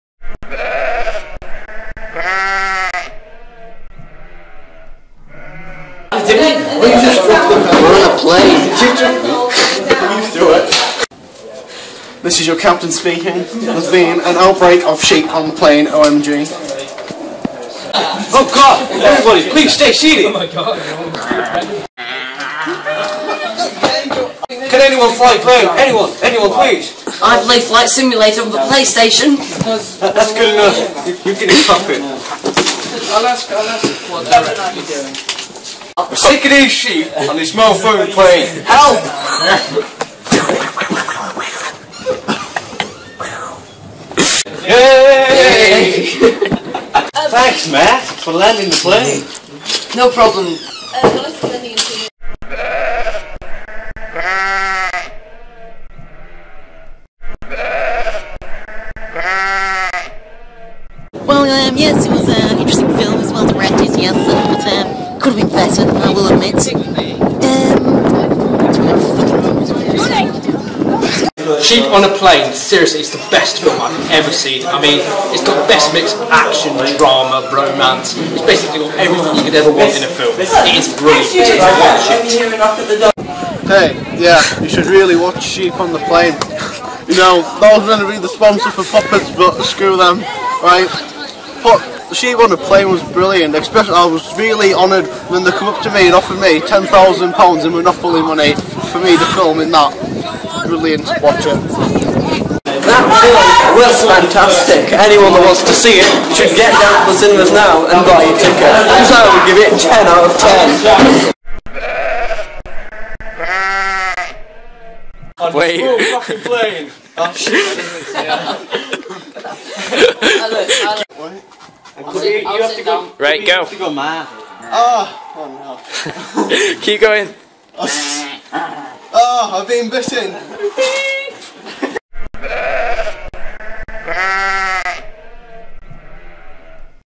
Sheep on a Plane
this is a short film we made in our english class, hope you enjoy it